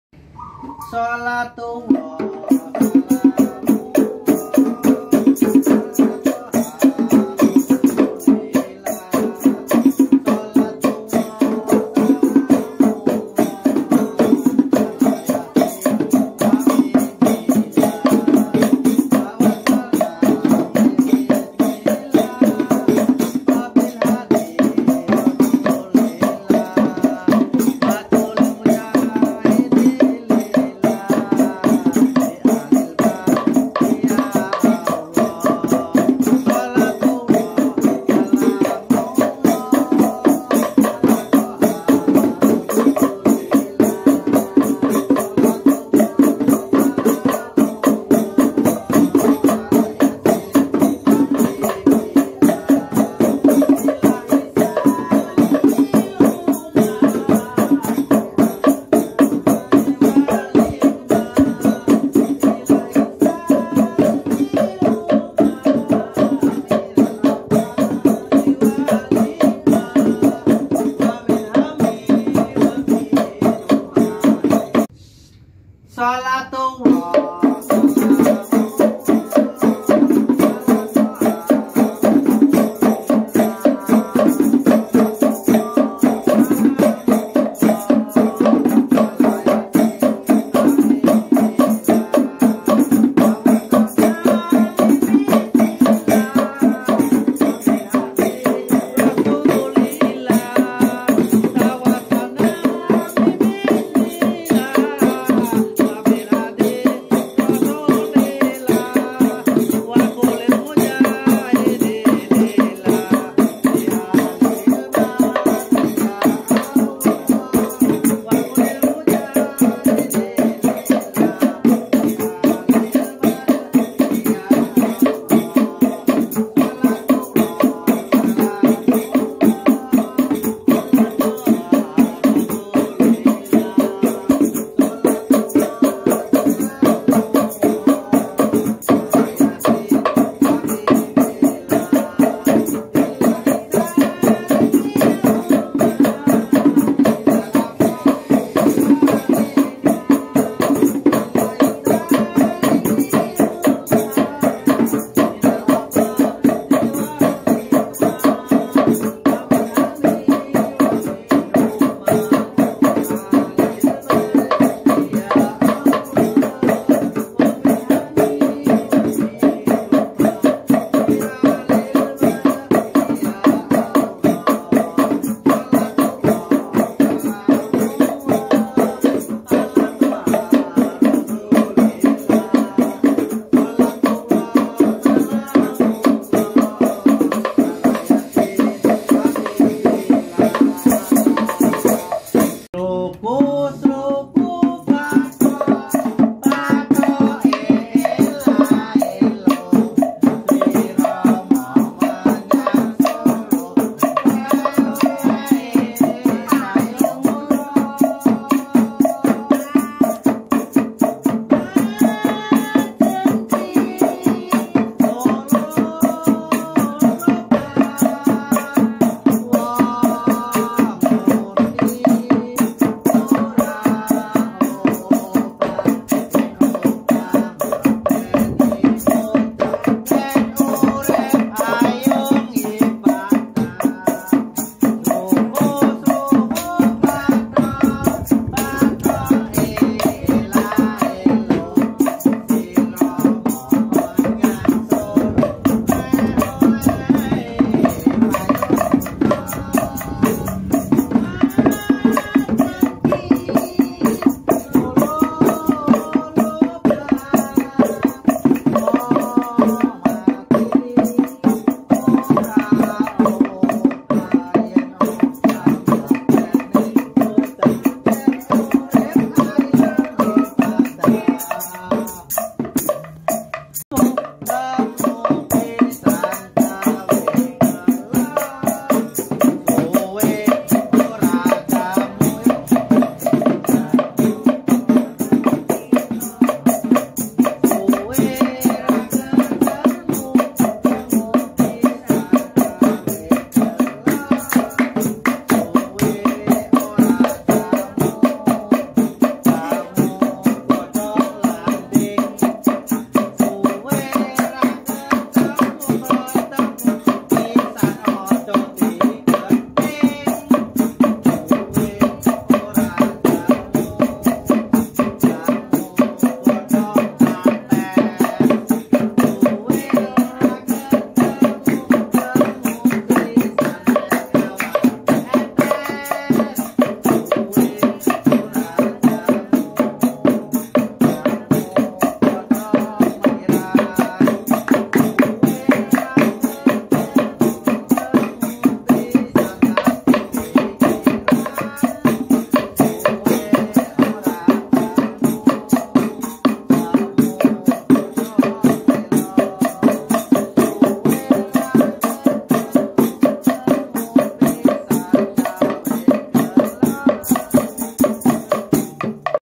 murid rumah hebat fatonah berlatih musik perkusi
kegiatan ini dilakukan secara terus-menerus pada hari Sabtu tanggal 6 Februari. kegiatan yang dilakukan di rumah hebat Fathonah salah satunya kegiatan berlatih musik